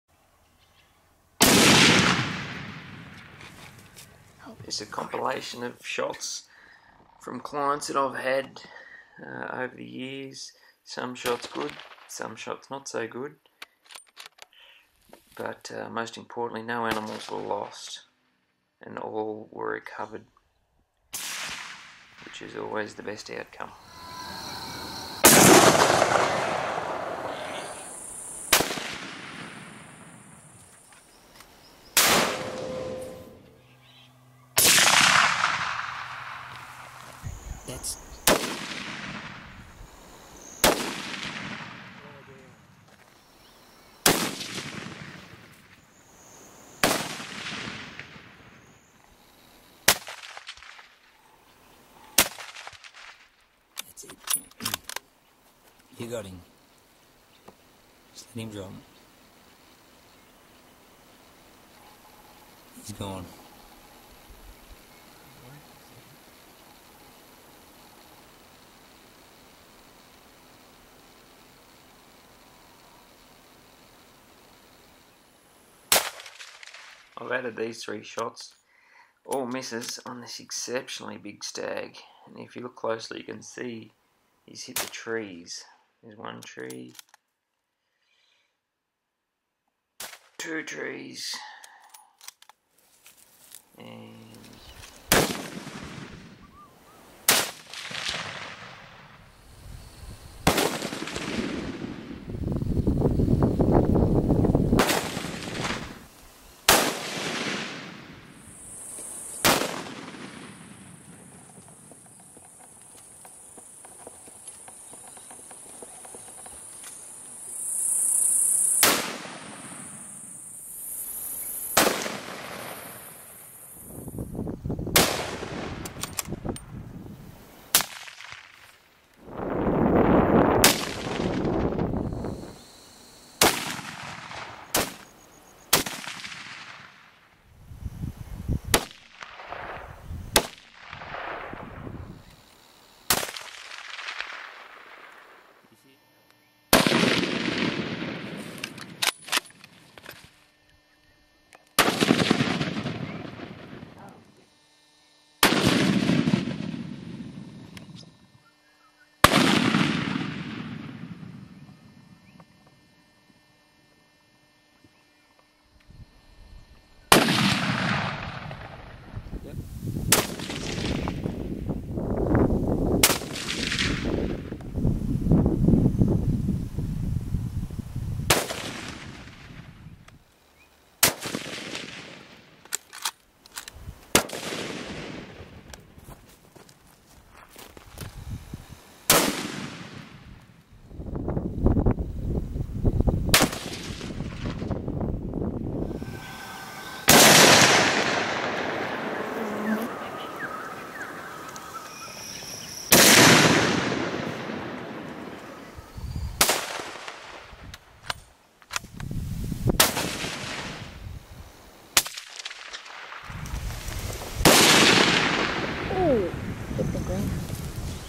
Kill shots 6. Deer. Chital Deer, Axis Deer.
Kill Shots 6. This is a short compilation clip of kill shots I've put together of hunters, I've always been behind the camera. Some shots good, some shots, not so good. This is hunting, real and true. No animals were lost. Every animal recovered.